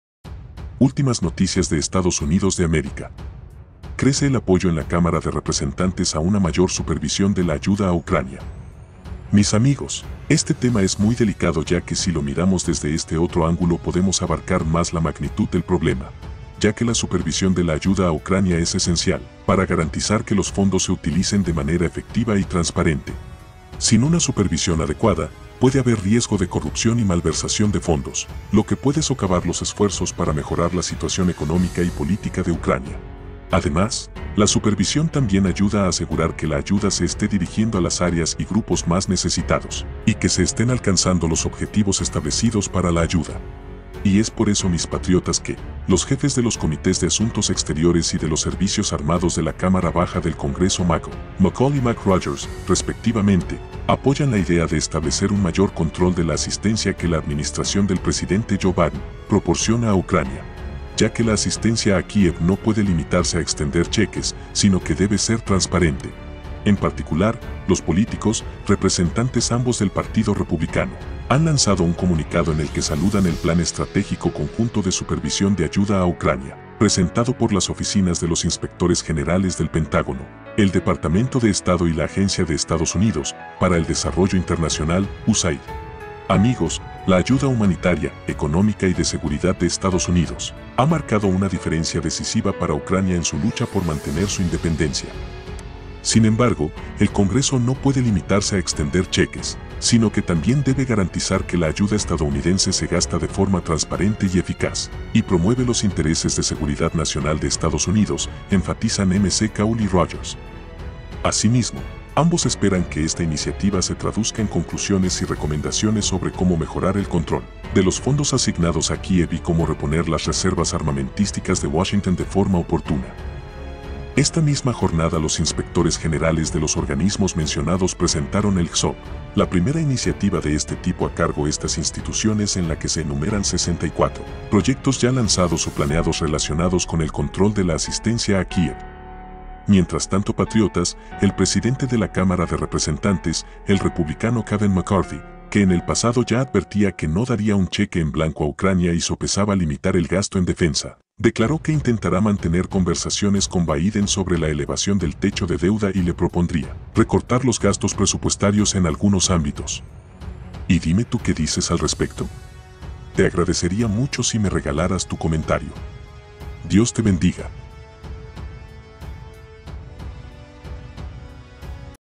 ULTIMAS NOTICIAS DE ESTADOS UNIDOS DE AMÉRICA CRECE APOYO EN LA CÁMARA DE REPRESENTANTES 19 DE ENERO
NOTICIAS DE ESTADOS UNIDOS DE AMERICA  TRUM RETOMA EL PODER 19 DE ENERO 2023
politica,joe biden,telemundo,noticias telemundo,estados unidos,donald trump,noticias de hoy,toma del